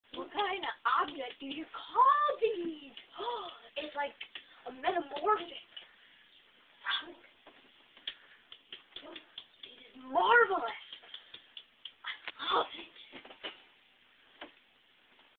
Boob tube dumber than dumb
When Bob finds this new thing called a light he cant figure out how it works or even what the crap it is!!!